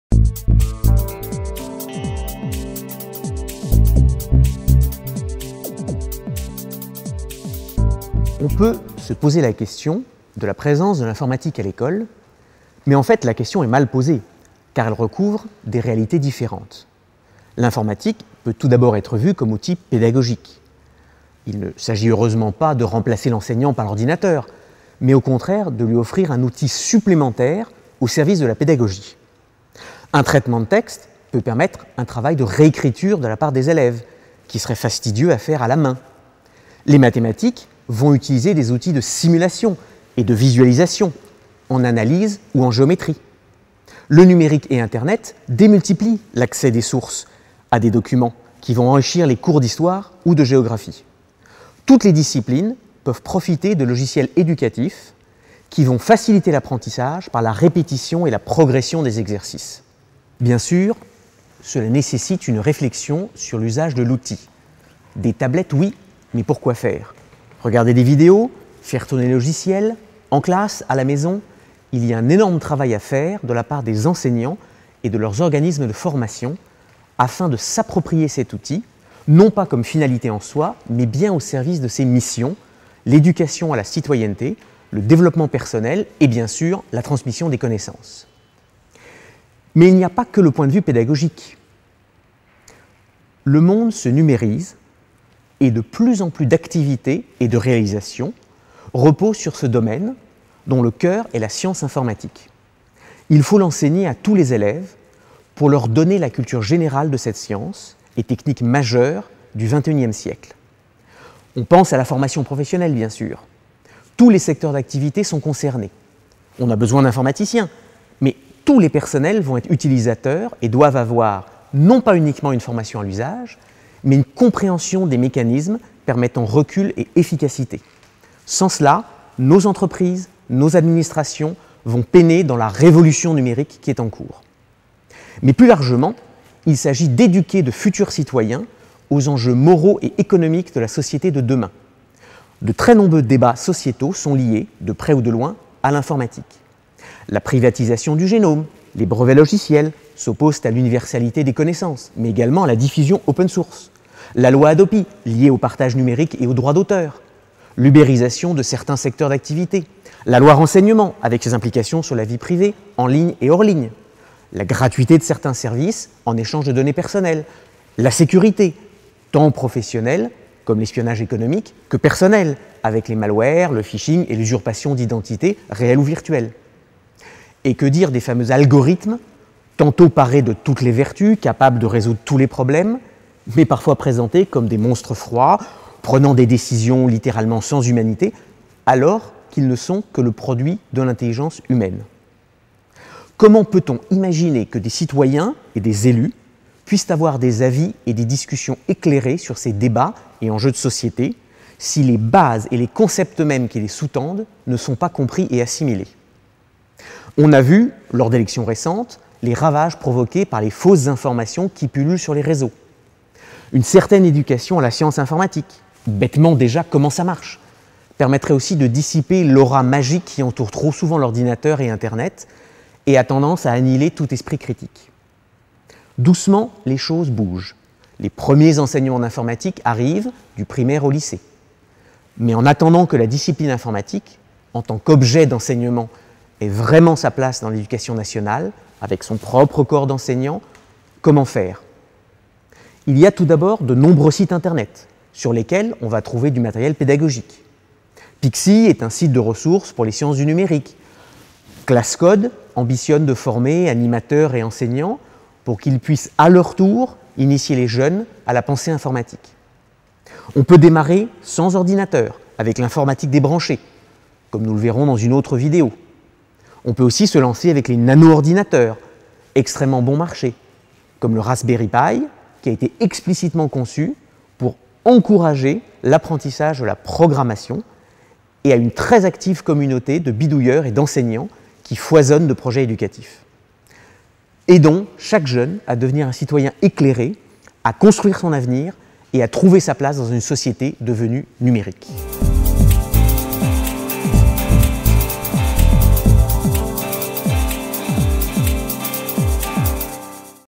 MOOC - Conversion numérique - Pourquoi l'informatique à l'école EL-1/6
MOOC "Éducation par la recherche : conversion numérique  à l'École".

Partie enjeux et méthodes de la recherche.

Emmanuel Lazard, Maitre de conférences en informatique (Paris-IX)